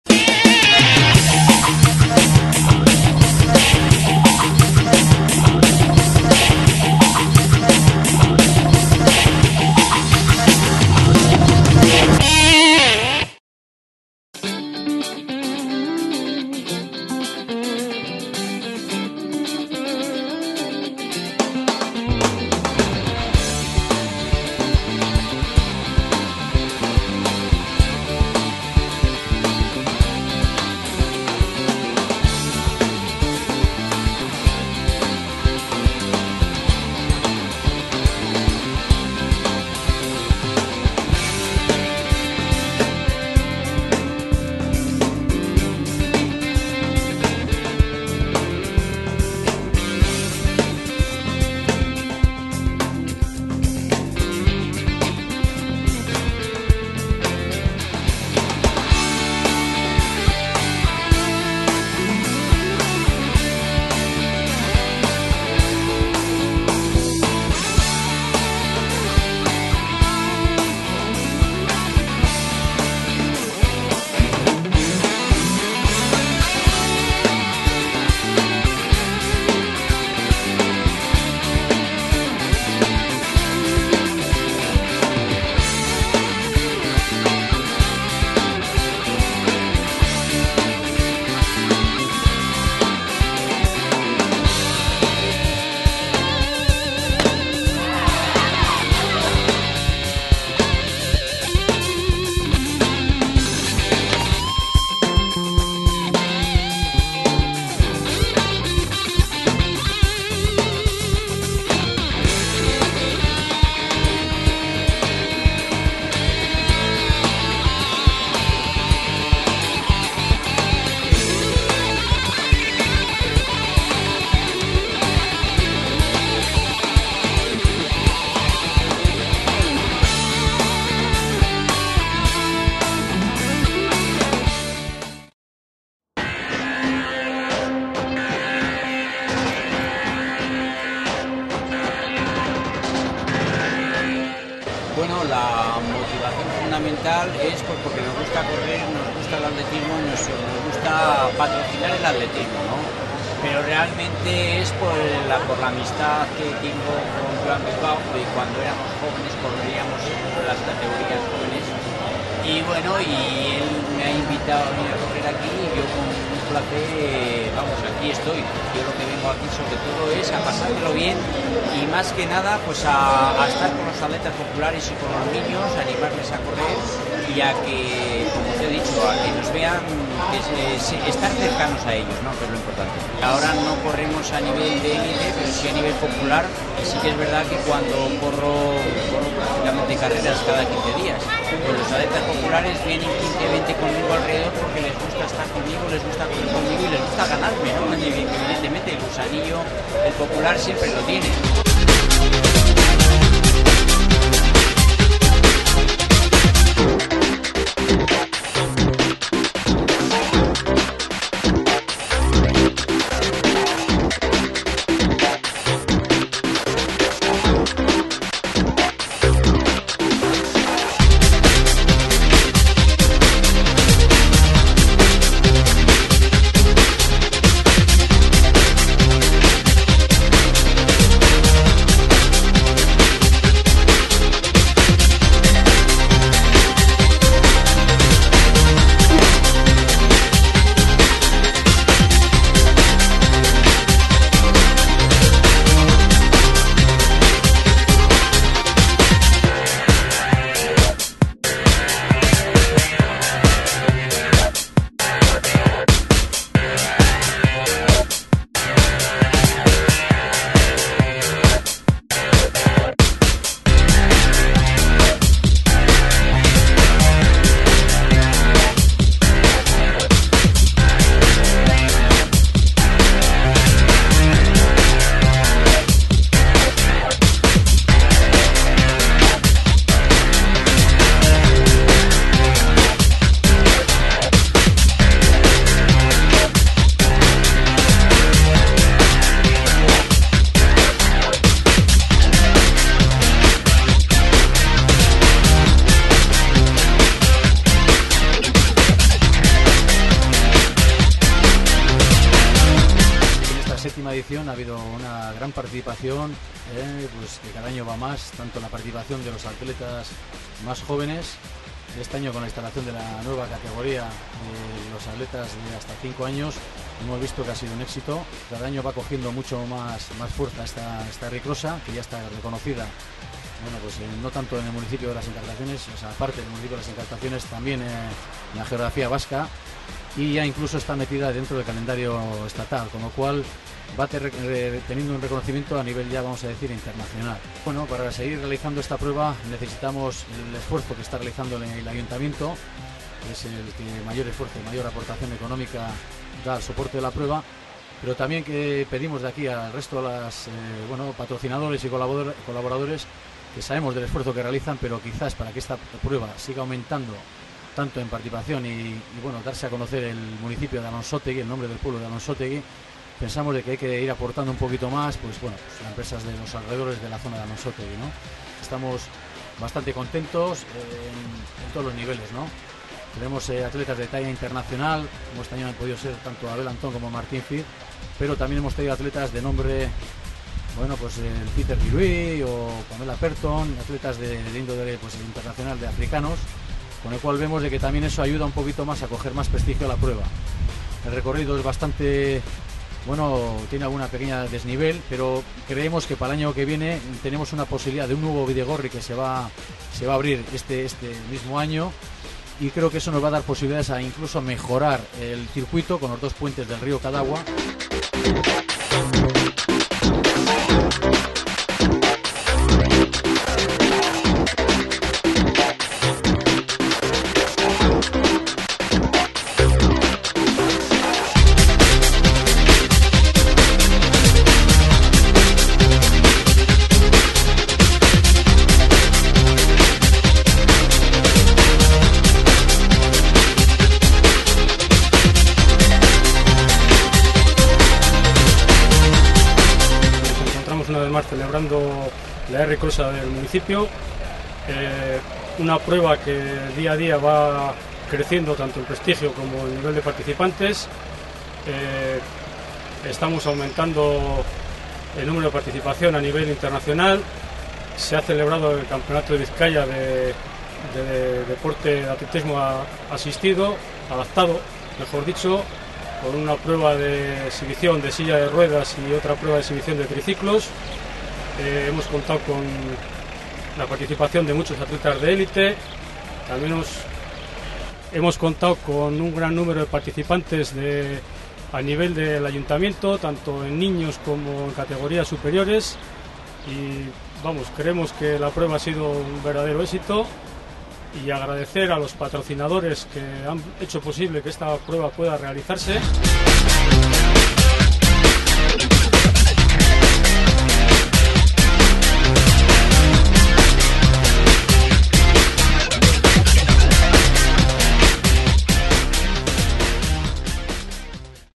Kross de Alonsotegi 2008
Cross Popular de Alonsotegui. Agradable ambiente familiar el que se brinda en esta carrera,  con un recorrido para no aburrirse y con disputas en todas las categorías.